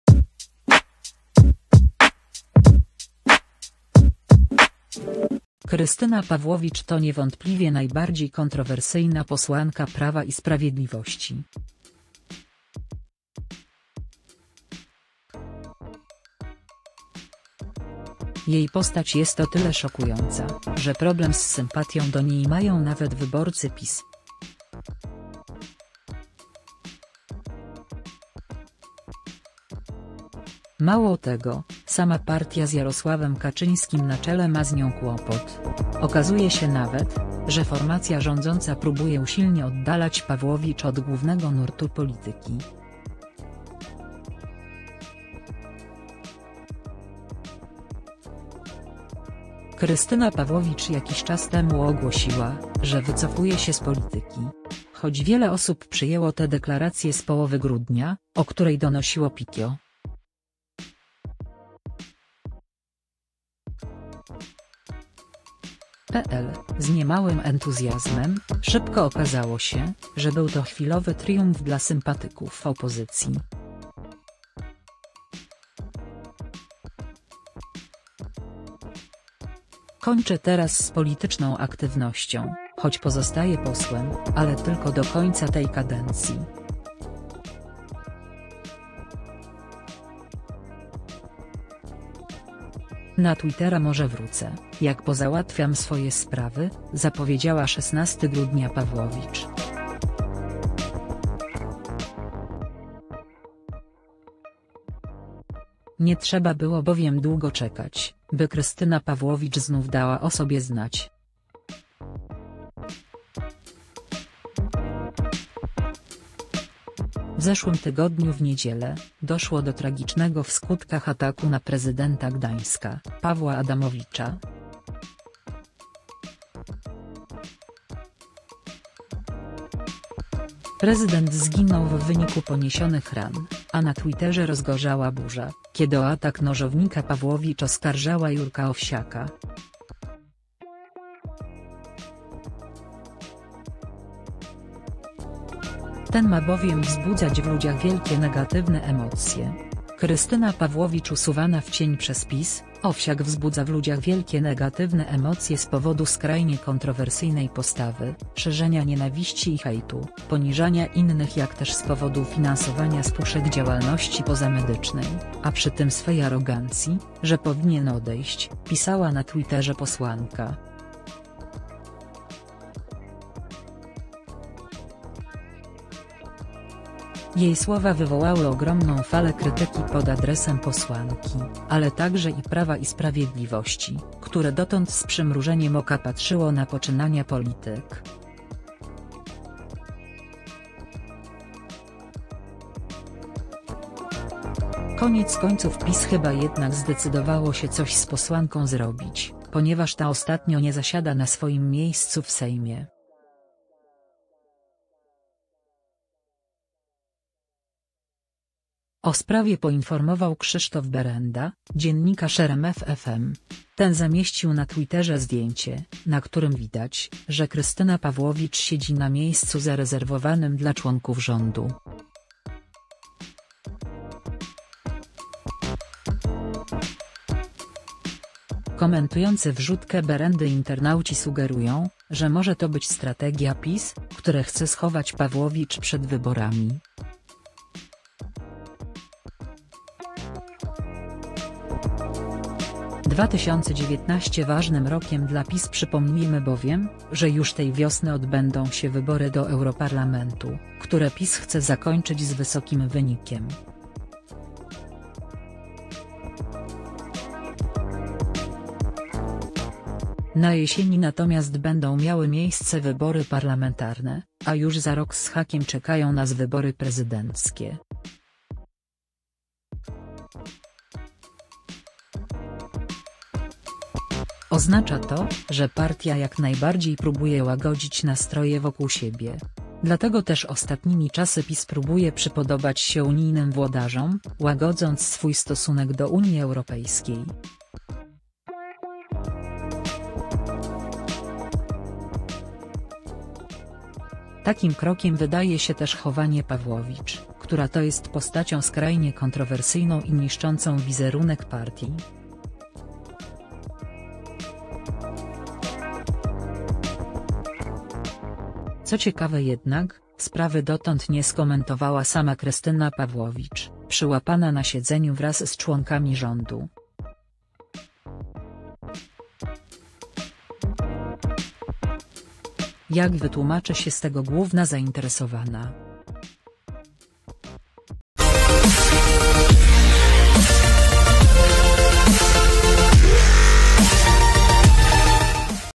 Pawłowicz przyłapana! Jak się z tego wytłumaczy? | Powiększenie
Dzięki za oglądanie! Jeśli lubisz wideo, zdobądź je i udostępnij znajomym. Kliknij tutaj, Krystyna Pawłowicz to niewątpliwie najbardziej kontrowersyjna posłanka Prawa i Sprawiedliwości. Jej postać jest o tyle szokująca, że problem z sympatią do niej mają nawet wyborcy PiS. Mało tego – sama partia z Jarosławem Kaczyńskim na czele ma z nią kłopot. Okazuje się nawet, że formacja rządząca próbuje usilnie oddalać Pawłowicz od głównego nurtu polityki. Krystyna Pawłowicz jakiś czas temu ogłosiła, że wycofuje się z polityki. Choć wiele osób przyjęło tę deklarację z połowy grudnia, o której donosiło Pikio.pl, z niemałym entuzjazmem, szybko okazało się, że był to chwilowy triumf dla sympatyków opozycji. – Kończę teraz z polityczną aktywnością, choć pozostaję posłem, ale tylko do końca tej kadencji. Na Twittera może wrócę, jak pozałatwiam swoje sprawy – zapowiedziała 16 grudnia Pawłowicz.    Nie trzeba było bowiem długo czekać, by Krystyna Pawłowicz znów dała o sobie znać. W zeszłym tygodniu w niedzielę, doszło do tragicznego w skutkach ataku na prezydenta Gdańska, Pawła Adamowicza. Prezydent zginął w wyniku poniesionych ran, a na Twitterze rozgorzała burza, kiedy o atak nożownika Pawłowicz oskarżała Jurka Owsiaka. Ten ma bowiem wzbudzać w ludziach „wielkie negatywne emocje”.Krystyna Pawłowicz usuwana w cień przez PiS? – Owsiak wzbudza w ludziach wielkie negatywne emocje z powodu skrajnie kontrowersyjnej postawy, szerzenia nienawiści i hejtu, poniżania innych jak też z powodu finansowania z puszek działalności „pozamedycznej”, a przy tym swej arogancji, że powinien odejść – pisała na Twitterze posłanka. Jej słowa wywołały ogromną falę krytyki pod adresem posłanki, ale także i Prawa i Sprawiedliwości, które dotąd z przymrużeniem oka patrzyło na poczynania polityk. Koniec końców PiS chyba jednak zdecydowało się coś z posłanką zrobić, ponieważ ta ostatnio nie zasiada na swoim miejscu w Sejmie.  O sprawie poinformował Krzysztof Berenda, dziennikarz RMF FM. Ten zamieścił na Twitterze zdjęcie, na którym widać, że Krystyna Pawłowicz siedzi na miejscu zarezerwowanym dla członków rządu. Komentujący wrzutkę Berendy internauci sugerują, że może to być strategia PiS, które chce „schować” Pawłowicz przed wyborami. 2019 ważnym rokiem dla PiS Przypomnijmy bowiem, że już tej wiosny odbędą się wybory do Europarlamentu, które PiS chce zakończyć z wysokim wynikiem. Na jesieni natomiast będą miały miejsce wybory parlamentarne, a już za rok z hakiem czekają nas wybory prezydenckie. Oznacza to, że partia jak najbardziej próbuje łagodzić nastroje wokół siebie. Dlatego też ostatnimi czasy PiS próbuje przypodobać się unijnym włodarzom, łagodząc swój stosunek do Unii Europejskiej. Takim krokiem wydaje się też „chowanie” Pawłowicz, która to jest postacią skrajnie kontrowersyjną i niszczącą wizerunek partii. Co ciekawe jednak, sprawy dotąd nie skomentowała sama Krystyna Pawłowicz, przyłapana na siedzeniu wraz z członkami rządu. Jak wytłumaczy się z tego główna zainteresowana?